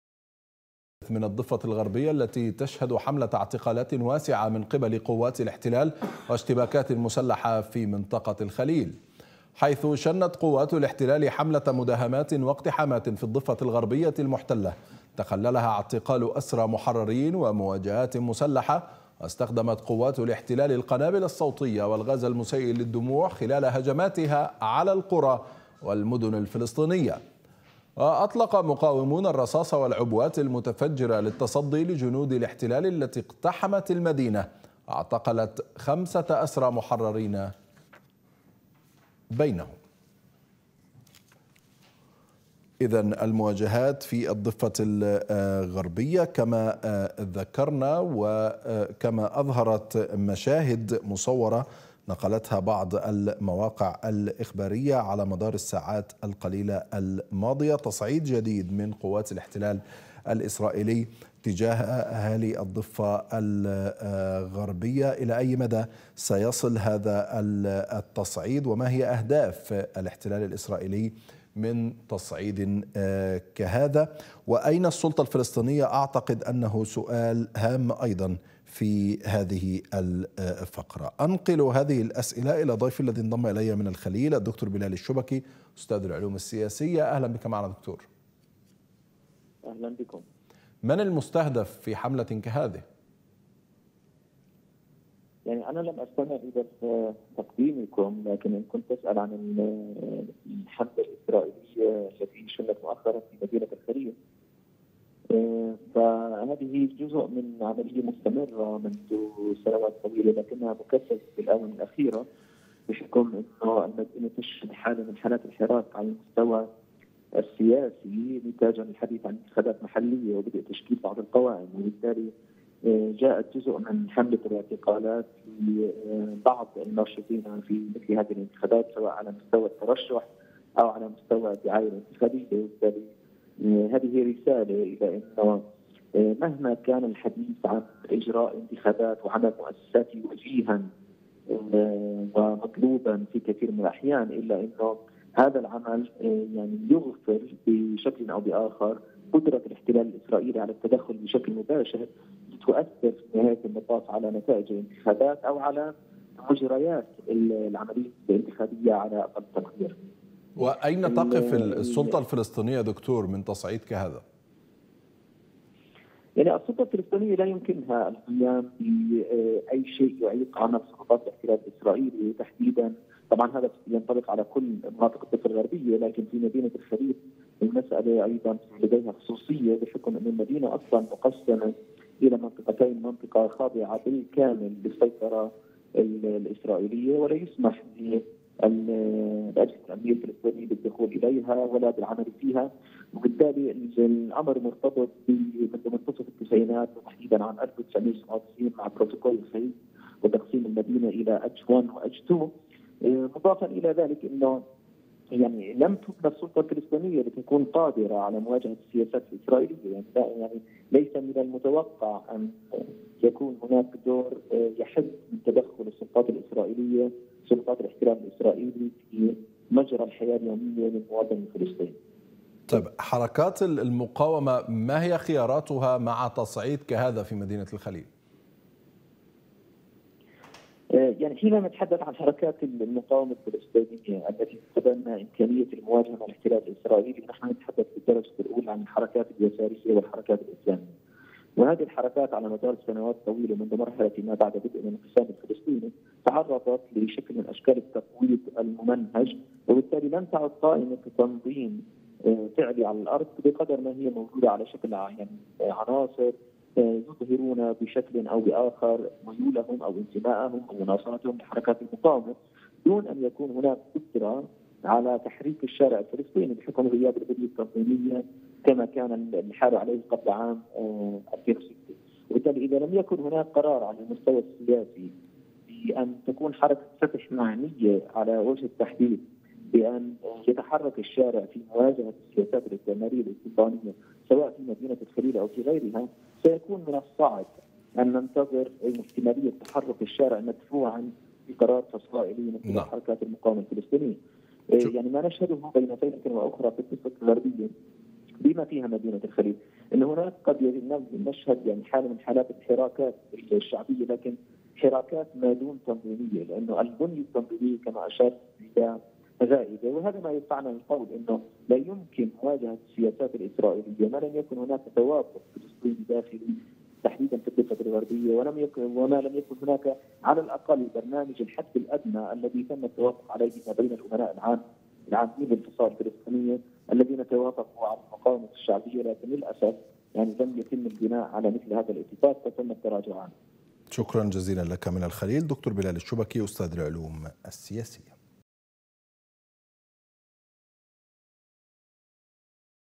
ما هو موقف السلطة الفلسطينية من التصعيد الإسرائيلي الأخير بالضفة؟
اشتباكات مسلحة في الخليل والاحتلال الإسرائيلي يقتحم مواقع عدة في الصفة الغربية، ما الأسباب؟
لماذا تشن سلطات الاحتلا الاسرائيلية حملة اعتقالات واسعة لأسرى محررين؟
ما خيارات الفلسطينيين تجاه الانتهاكات الإسرائيلية في الضفة الغربية؟

أضواء على الأحداث | 14 مارس 2022 | أسامة جاويش يستضيف أستاذ العلوم السياسية، بلال الشوبكي.